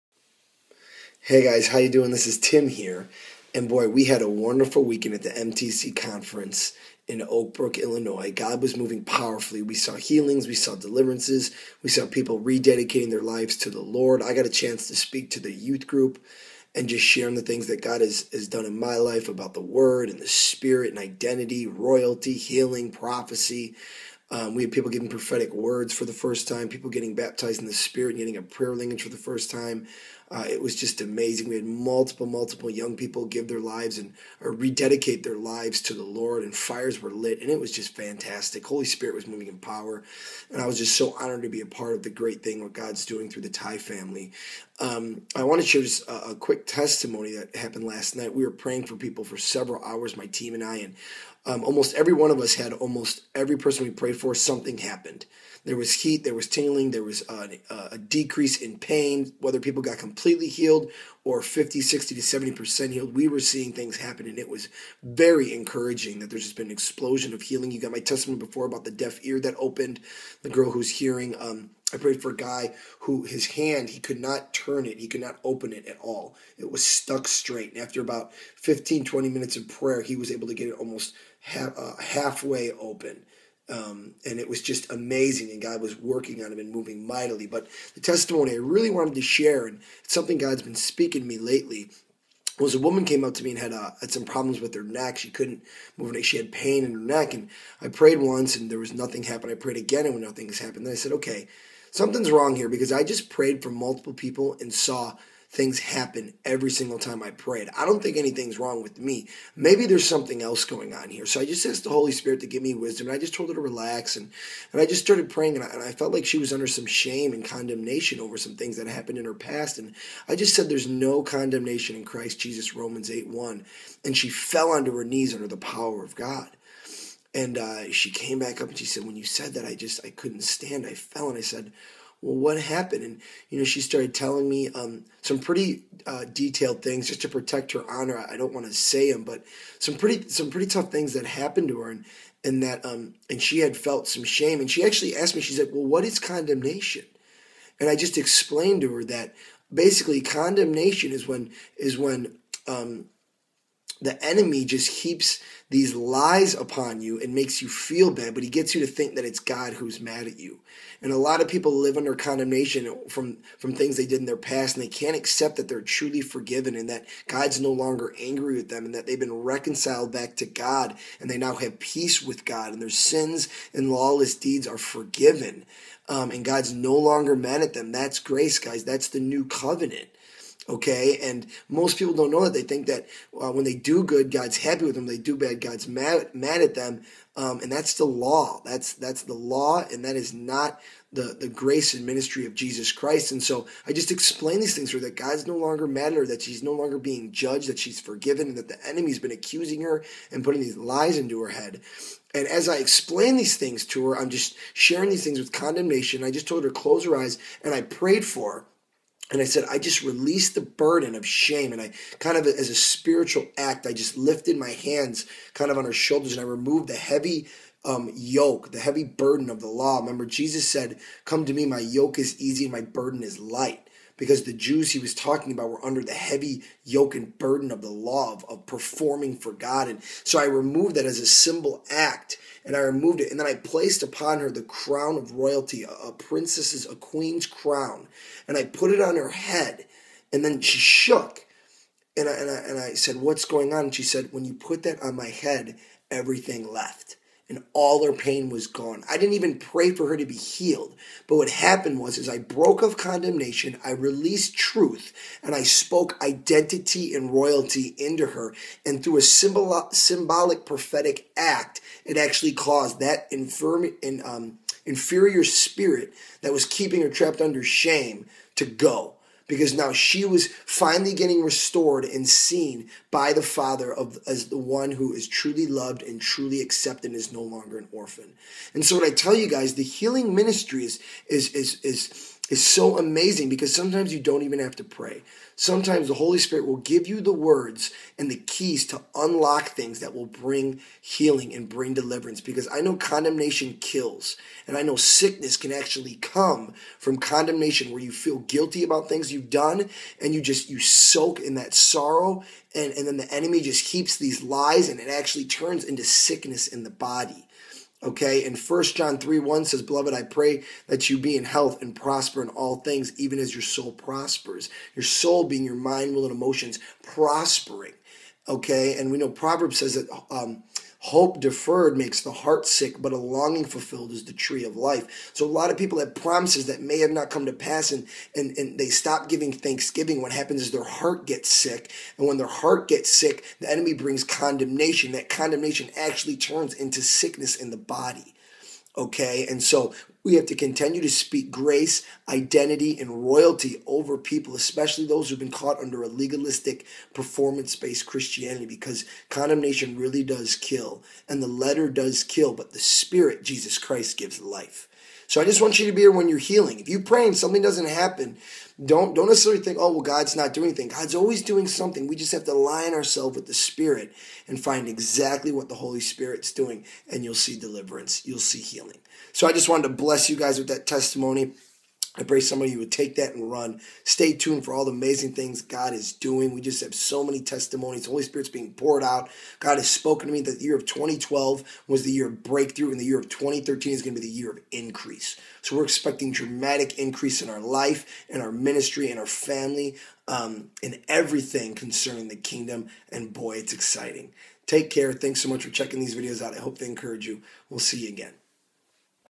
Healing Revelation!
Sometimes we pray for healing and nothing seems to happen however in the Spirit realm God is moving powerfully. Jesus prayed many different ways and always was led by the Spirit! This is the key to see greater results in the healing ministry